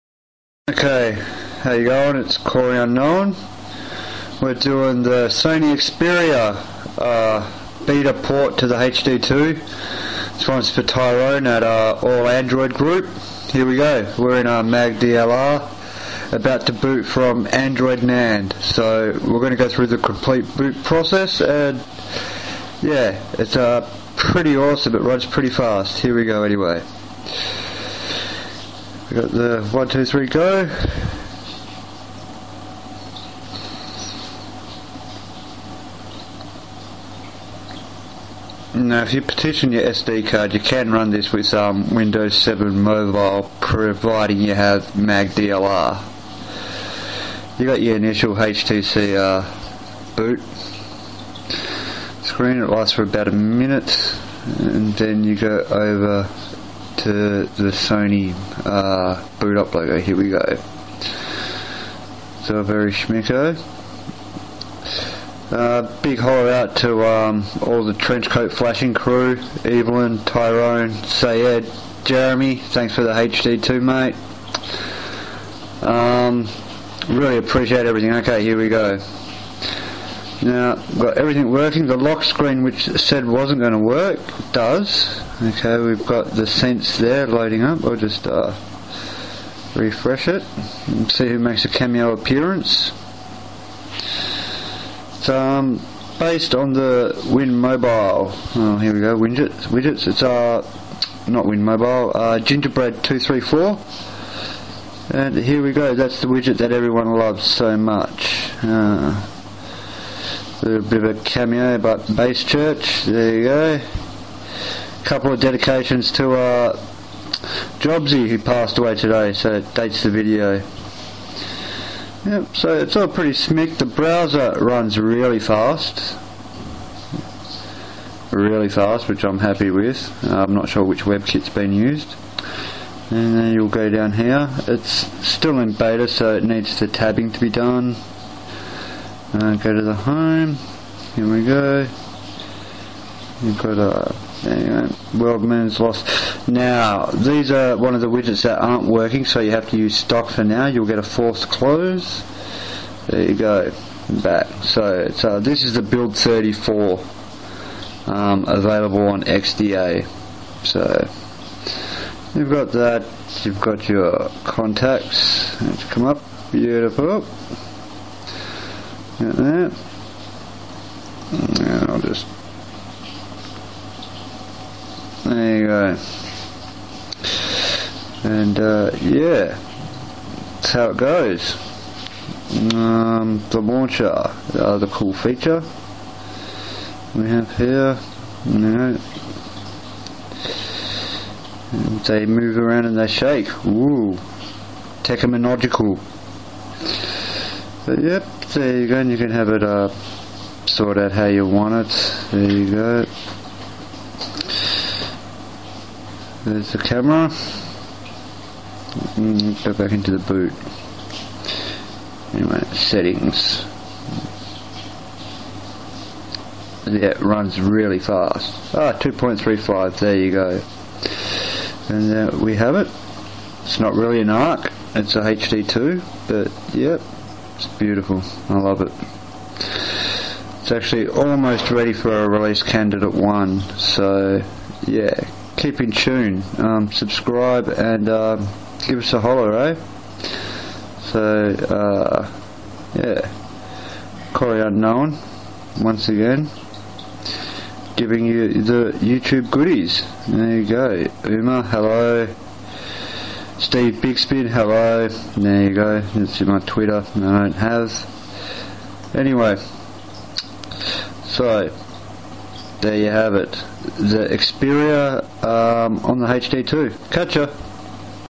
Sony Xperia ARC beta build 34
A very impressive port of sonys xperia arcto the htc hd2 . Based on GB2.3.5 and incredibly fast .. links are up on XDA website.